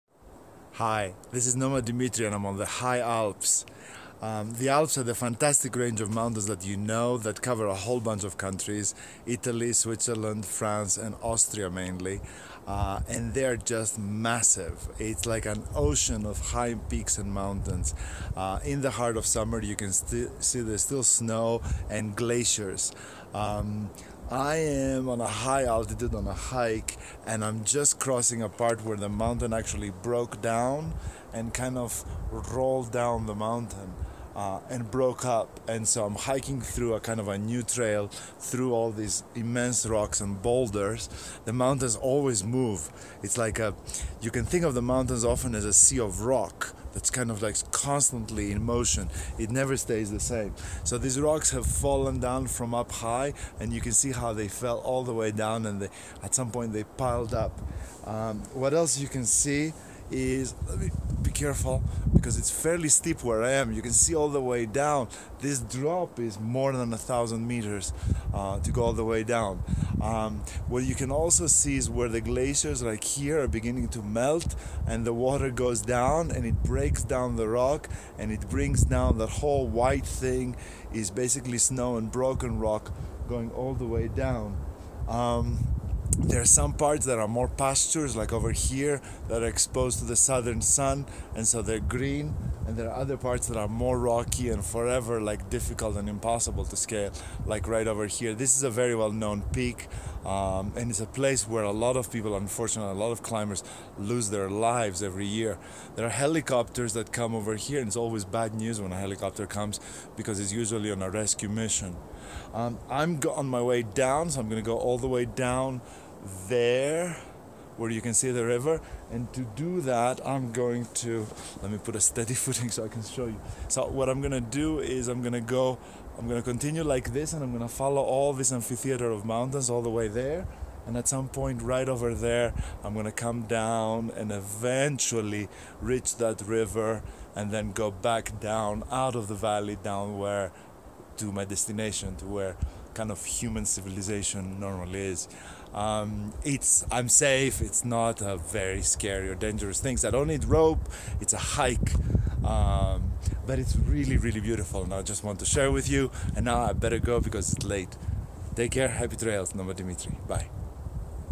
The Alps are so much younger than the Rockies
The magnificent ocean of rock that comprises the Alps looks much older than it is!  In fact, compared to several North American mountain ranges, they are absolute toddlers (measuring in the millions versus *billions* of years of age).

This is also, perhaps, what makes them so fascinating to hike: they are always moving & changing.  The nice trail you hiked on last spring has, by this autumn, collapsed to the bottom of the valley 3000 feet below.

This instability is also what makes them deadly: every summer, the grim site of a helicopter searching for corpses of hikers is a regular sight.  Yet, we are drawn to the beauty of these wild spaces, the last ones left in old europe.